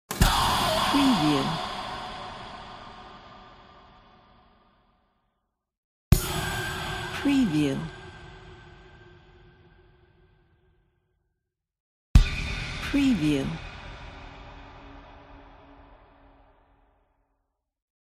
Trailer Impacts Sound Effects
They are tense, edgy and unsettling widescreen whooshy and screaming impacts, specially designed for your horror films, video games, trailer transitions, cinematic jump scares, intros, titles, cartoons, promos, sci-fi contents, documentaries, vlogs and many more. Feel free to drop them on your timeline today and you will be able to conquer the world with such an ease! Enjoy it!

3 items are available in the main ZIP.
I wish you many successful projects!